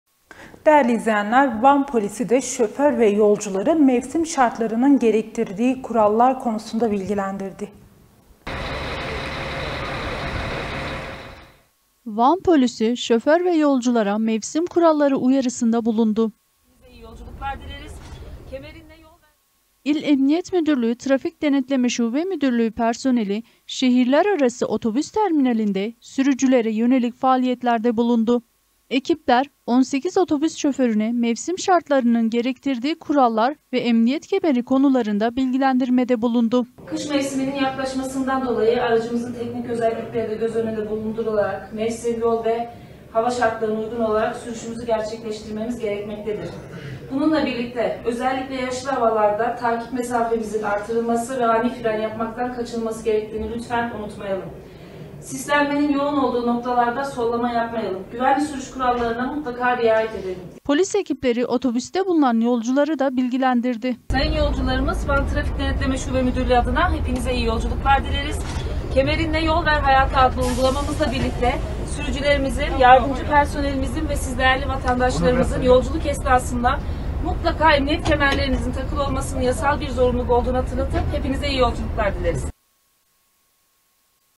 Van polisinden sürücü ve yolculara mevsim kuralları uyarısı
Van ve Bölgenin Televizyonu.. Halkın ve Hakkın Sesi..
twitter.comvangolutv
facebook.comvangolutv
instagram.comvangolurtv
youtube.comvangolutv